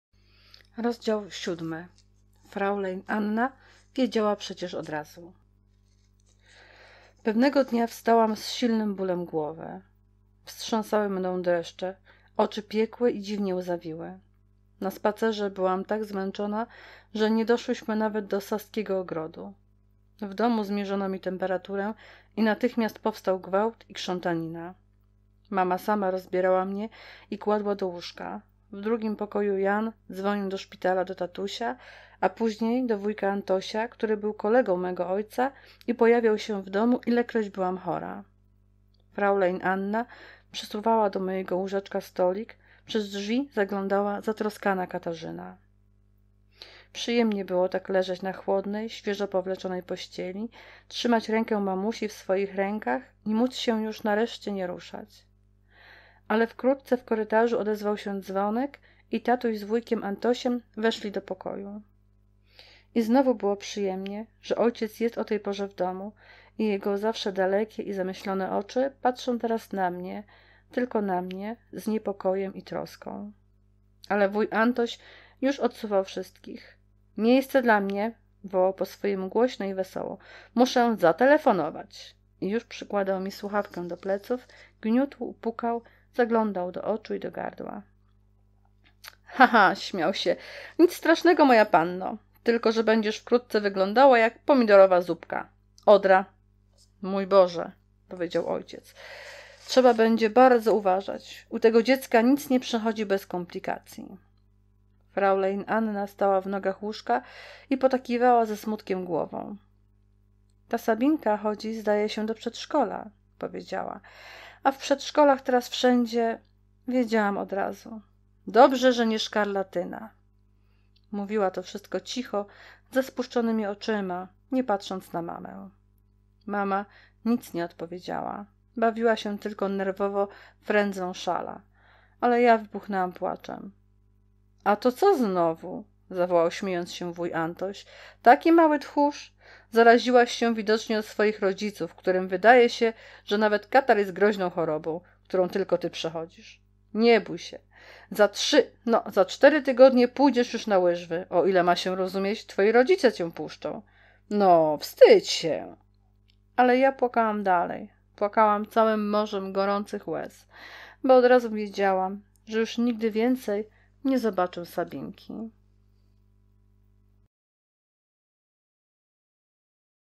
Halina Górska- Druga Brama - Audiobook rozdział 7
Mądra opowieść o trudnej przyjaźni dwóch dziewczynek, żyjących pod dwu stronach bramy podwórka: po jednaj mieszkają dzieci rodziców zamożnych i wykształconych, pomimo troskliwej opieki, jaką są otaczane, niezmiernie samotne w świecie dorosłych, po drugiej - dzieci proletariackie, właściwie nie będące nigdy dziećmi, przygniecione całkiem dorosłymi zmartwieniami i obowiązkami. Rzecz dzieje się w Warszawie, tuż po burzliwych wydarzeniach 1905 roku...Przepiękna i przejmująca powieść dla młodzieży..i nie tylko.